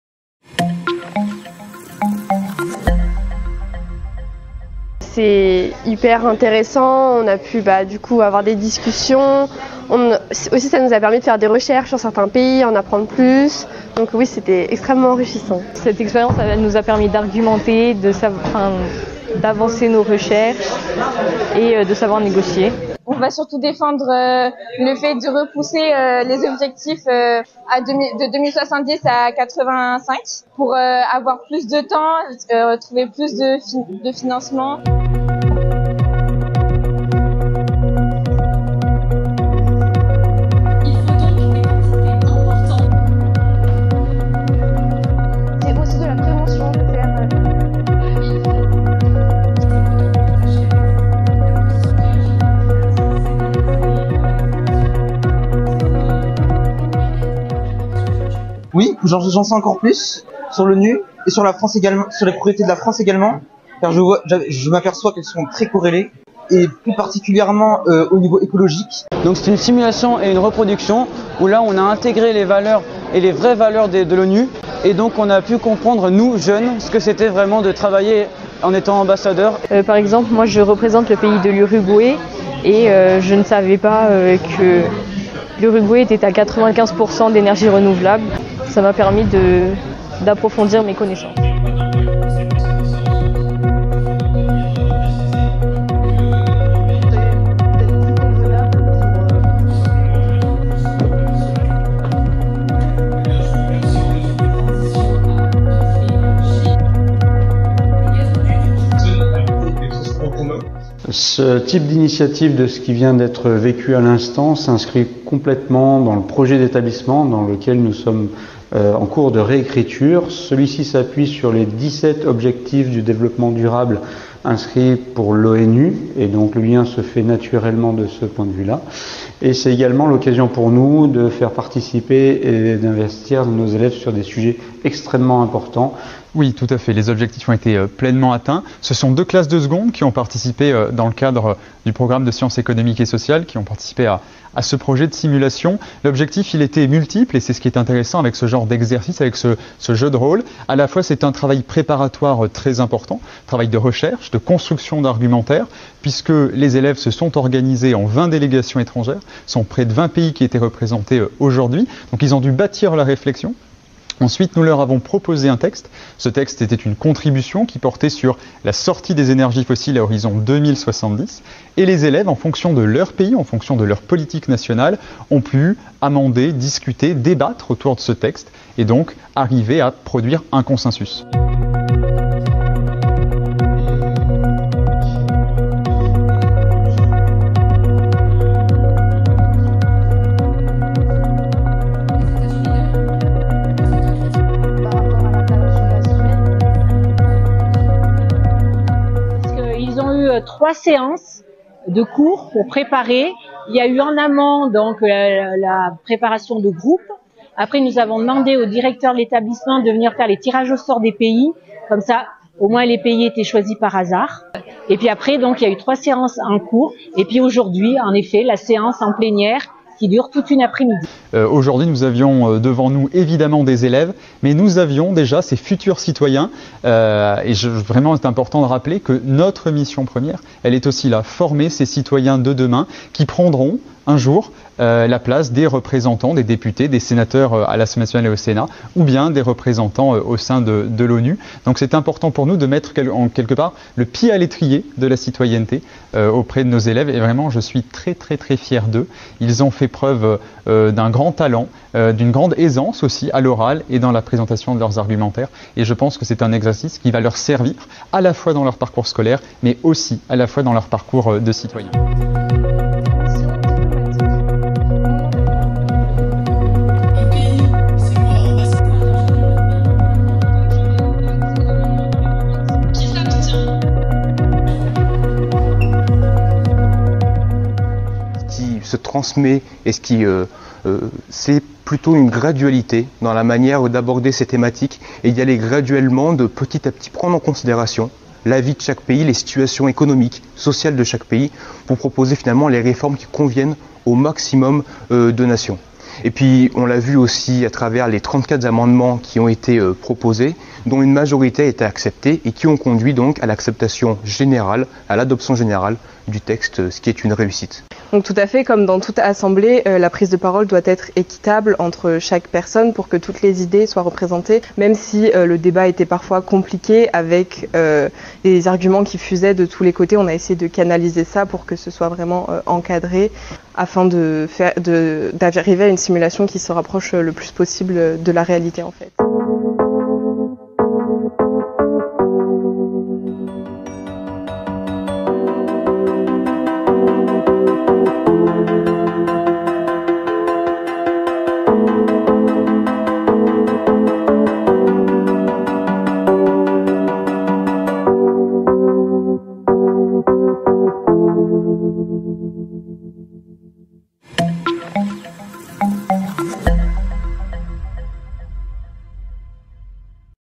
Une Simul'ONU organisée par le Lycée Saint-Maurice à Romans
A Romans-sur-Isère, 65 élèves du Lycée Saint-Maurice ont participé aux Simul'ONU. Lors de cette simulation de conférences des Nations Unies, les jeunes participants ont débattu autour du projet de résolution portant la sortie des énergies fossiles à l'horizon  2070.

RETOUR EN IMAGES SUR LA RESTITUTION